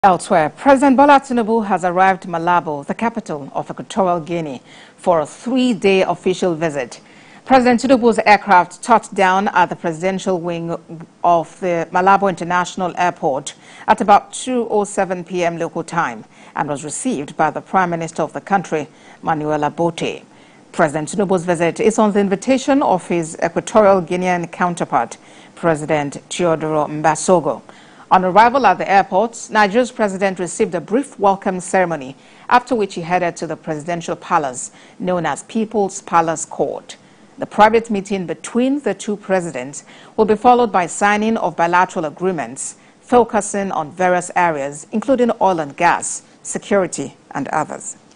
Nigeria, Equatorial Guinea To Sign Petroleum, Security Deals
President Bola Tinubu has arrived Malabo, the capital of Equatorial Guinea, for a three-day official visit.
President Tinubu's aircraft touched down at the Presidential Wing of the of the Malabo International Airport at about 2:07pm local time and was received by the Prime Minister of the country, Manuela Botey.
President Tinubu's visit is on the invitation of his Equatorial Guinean counterpart, President Teodoro Mbasogo